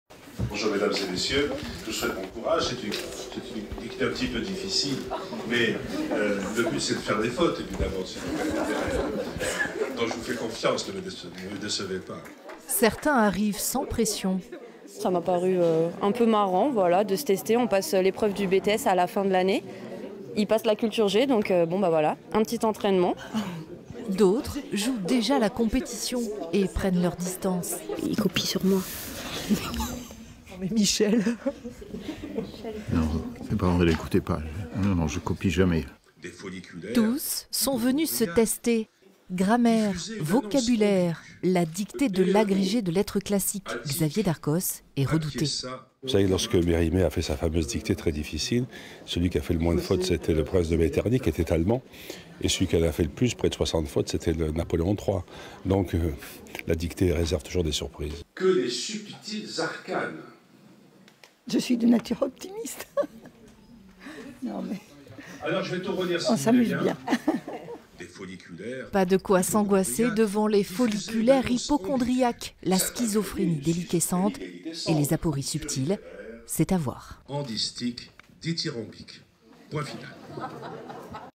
La dictée gourmande de Xavier Darcos
A l'occasion du 15 ème salon du livre Gourmand de Périgueux, il était proposé aux amateurs de mets et de mots une dictée préparée par l'ancien ministre de l'Education Nationale Xavier Darcos...